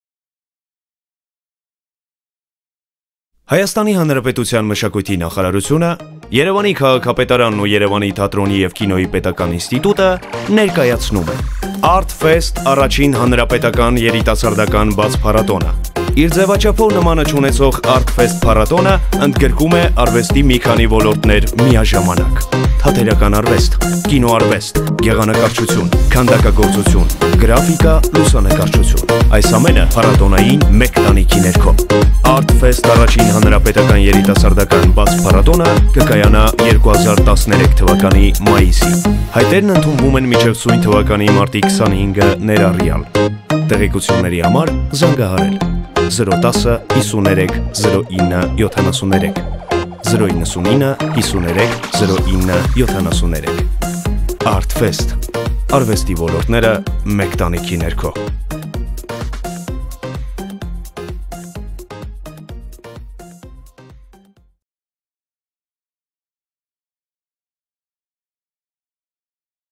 "ԱՐՏ-ՖԵՍՏ" ՀԱՅՏԵՐԻ ԸՆԴՈՒՆՈՒՄ
ՀՀ Մշակույթի նախարարությունը, Երևանի քաղաքապետարանն ու Երևանի թատրոնի և կինոյի պետական ինստիտուտի Ուսումնական թատրոնը 2013թ. մայիսին մեկնարկում են "ԱՐՏ-ՖԵՍՏ" առաջին հանրապետական երիտասարդական բաց փառատոնը` "Մեկ տանիքի ներքո" խորագրով, որը ներառում է արվեստի մի քանի ոլորտներ` Թատերարվեստ, Կինոարվեստ, Կերպարվեստ (գեղանկարչություն, քանդակագործություն, գրաֆիկա, լուսանկարչություն):